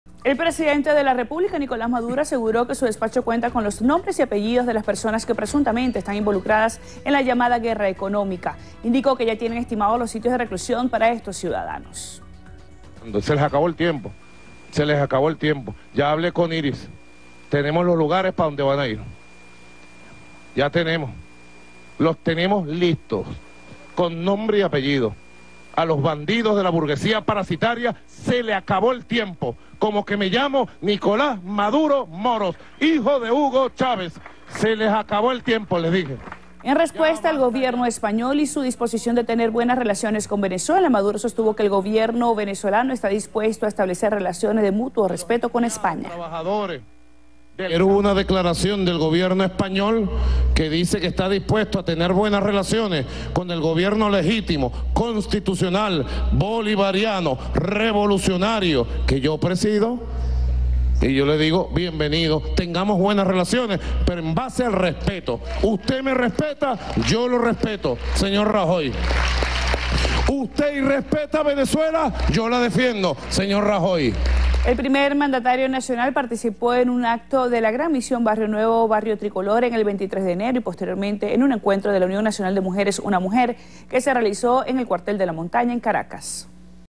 Maduro aseguró tener nombres de personas presuntamente involucradas en la guerra económica
El presidente de la República, Nicolás Maduro, aseguró este sábado que su despacho cuenta con los nombres y apellidos de las personas que presuntamente están involucradas en la llamada "guerra económica".